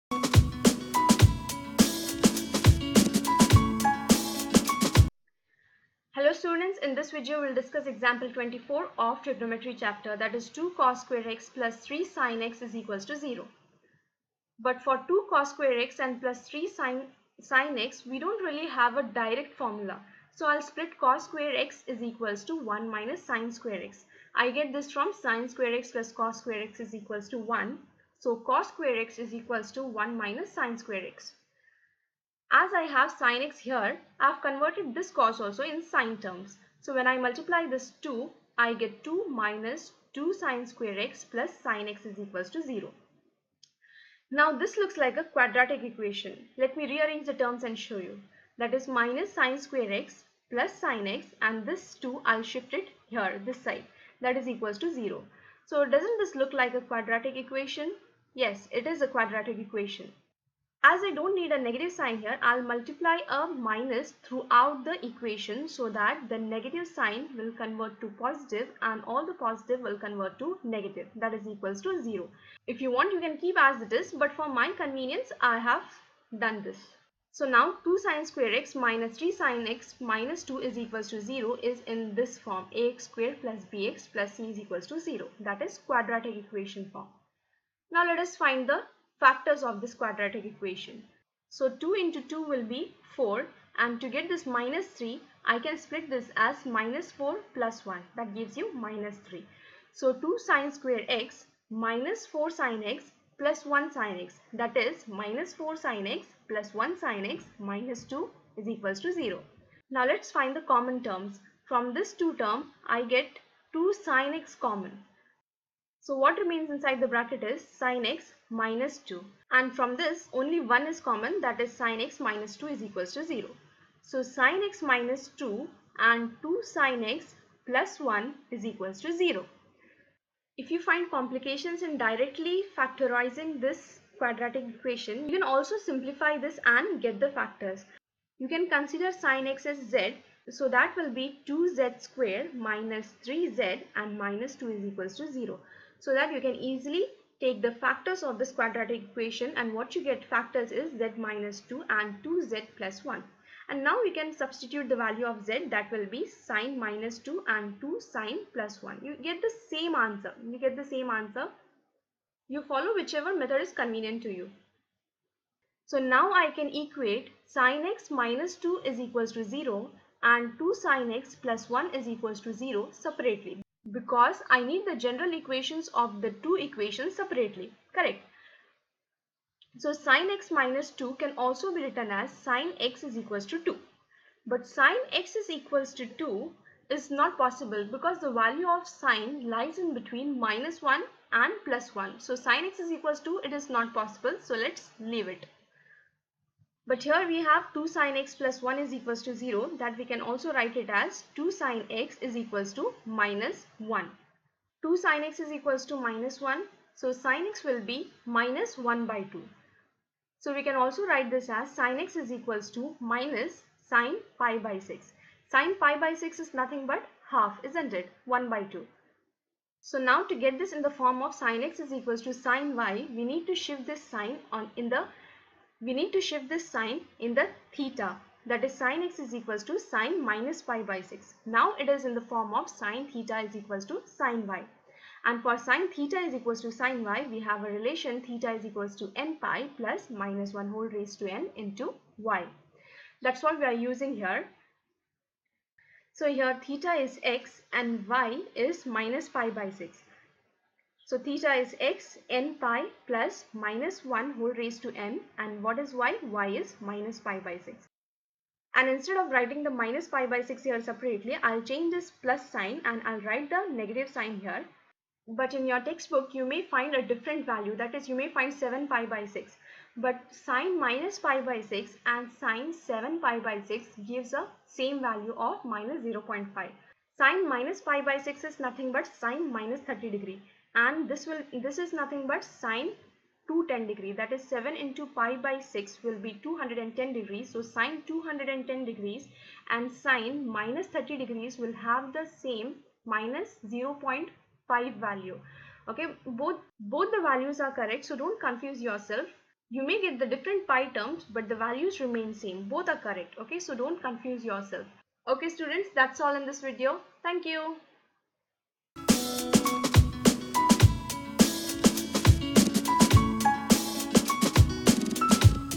Example 24 | Chapter 3 | Class 11 | Trigonometry Functions | NCERT math
trigonometric functions class 11 example 24.

 In this video i have discussed example 24 of class 11, chapter 3 (Trigonometric functions )

 Trigonometry is a branch of mathematics that studies relationships involving lengths and angles of triangles. The field emerged in the Hellenistic world during the 3rd century BC from applications of geometry to astronomical studies.

The main functions in trigonometry are Sine, Cosine and Tangent. They are simply one side of a right-angled triangle divided by another. For any angle "θ": (Sine, Cosine and Tangent are often abbreviated to sine, cos and tan.)

Trigonometry is defined as one of the branches of mathematics that deals with the relationships that involve lengths and also the angles of triangles.

In a simple words, Trigonometry is the study of triangles.

 Happy Learning!!!